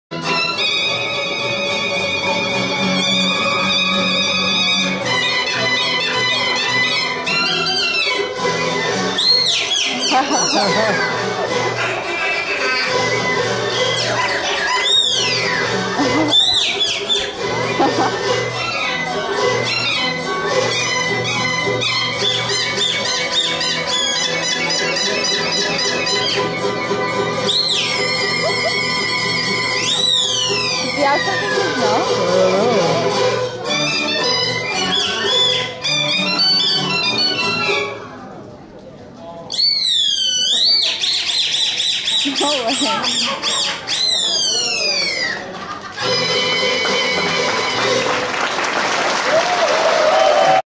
Chirping Man
This guy sings by chirping like a bird.